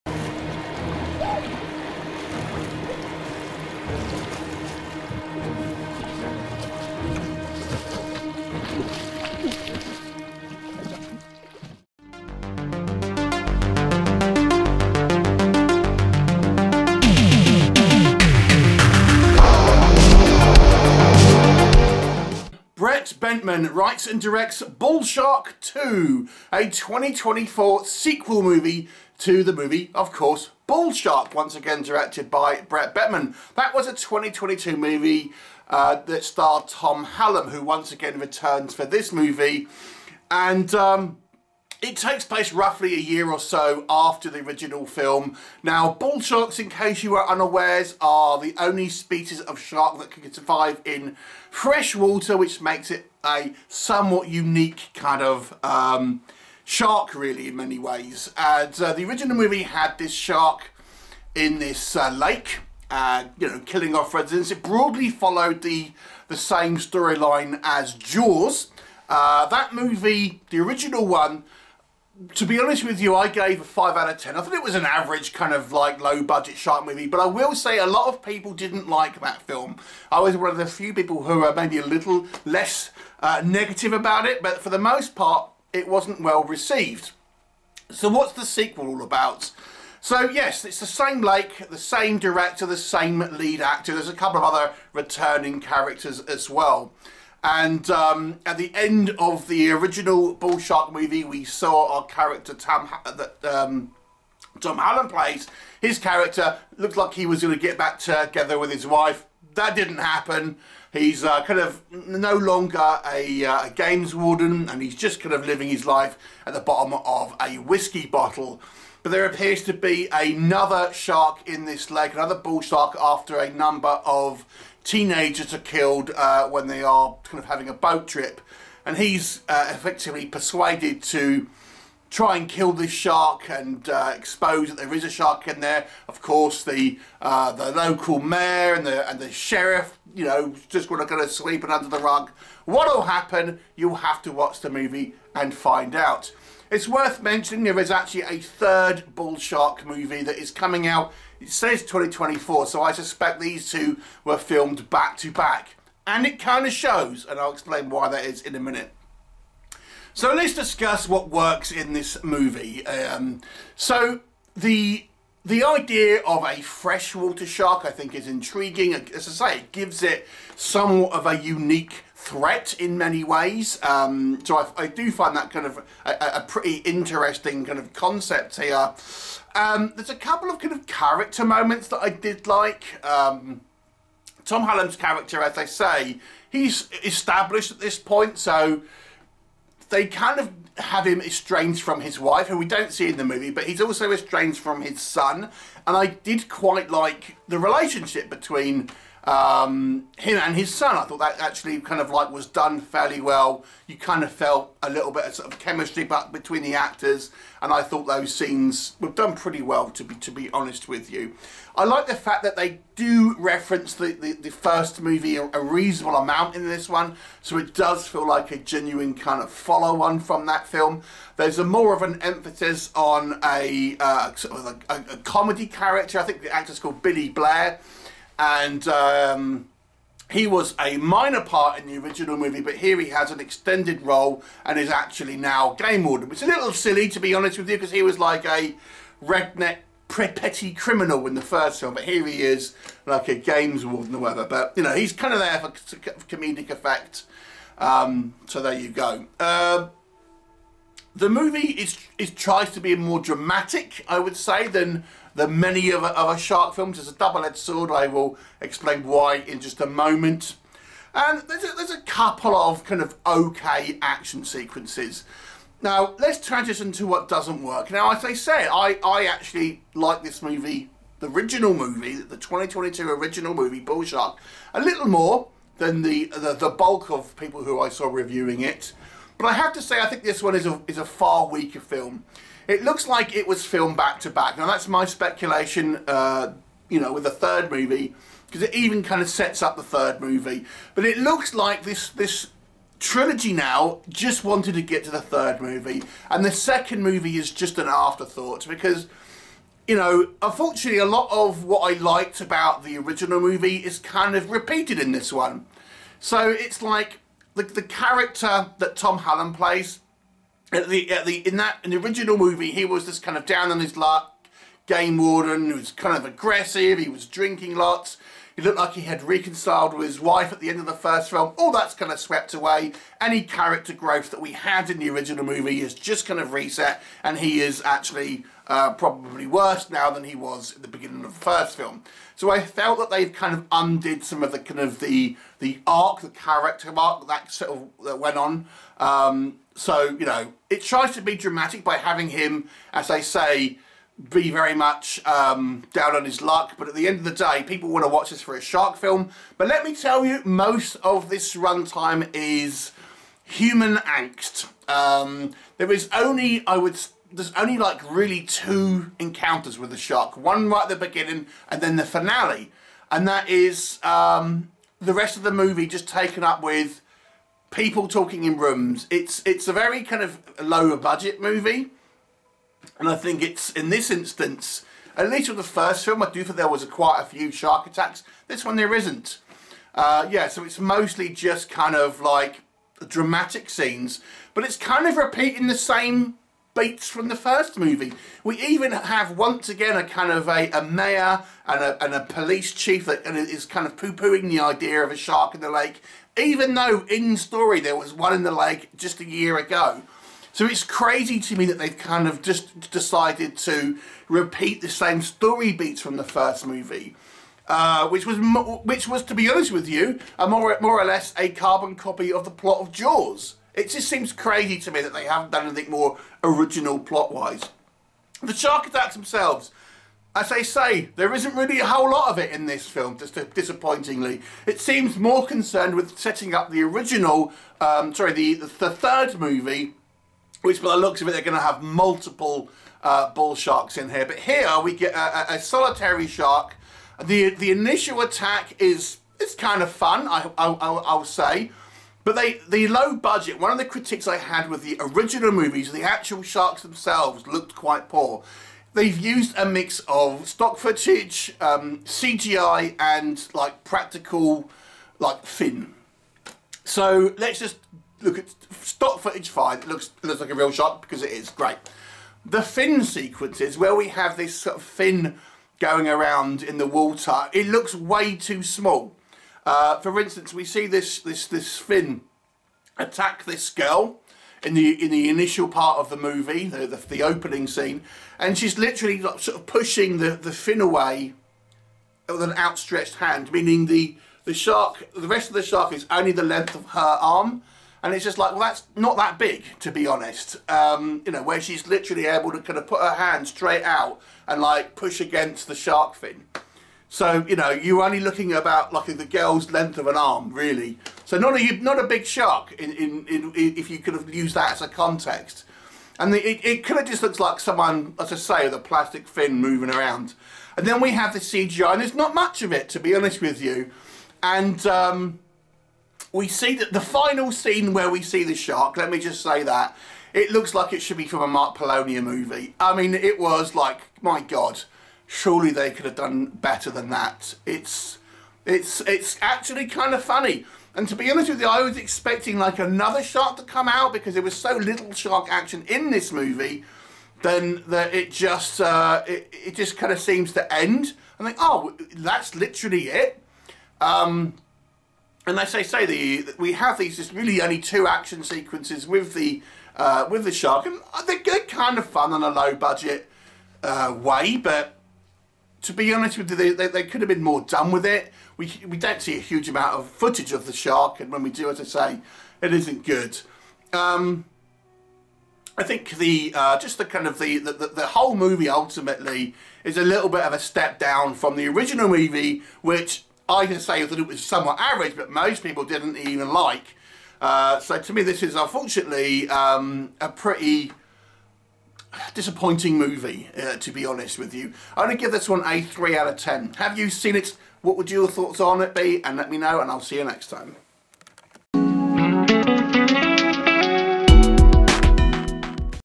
BULL SHARK 2 ( 2024 Thom Hallum ) Man Eating Sharks in a Lake Horror Movie Review
After the events of the first film, a second Bull Shark begins feasting on unsuspecting victims in a Texas lake.